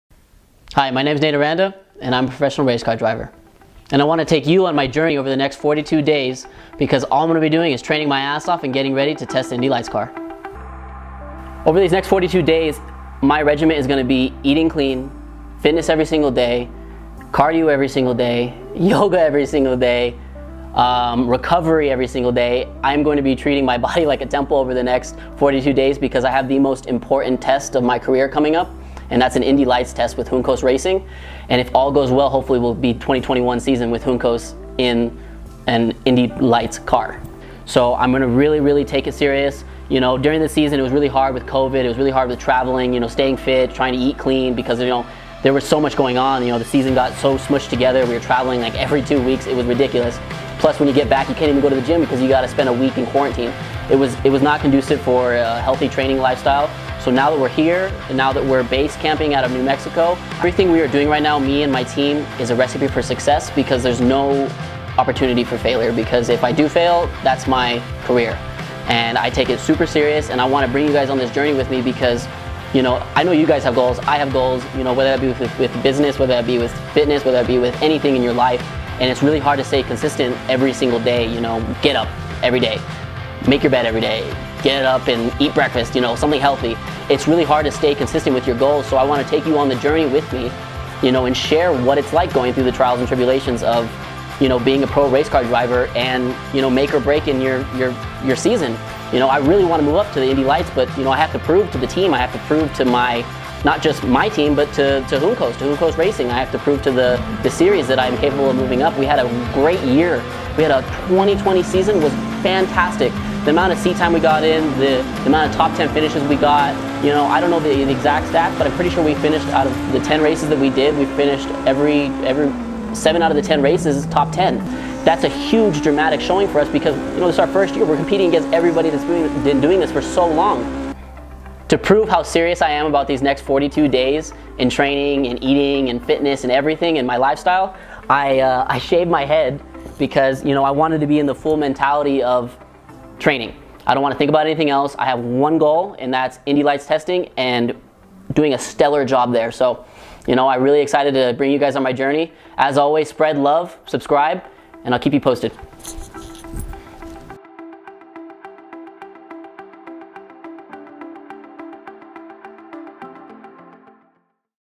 42 days of HARD TRAINING to compete in the Indy Lights Series
I wanted to share my journey over the next few weeks of me getting prepared for the Indy Lights testing in Florida. I had a really great season in 2020 in Indy Pro 2000 so I am going to train as hard as I can to prove I belong in the Indy Lights series. This is going to be a hard journey, but I am ready to prove to everyone that I belong. Join me over the next few weeks as I prepare myself.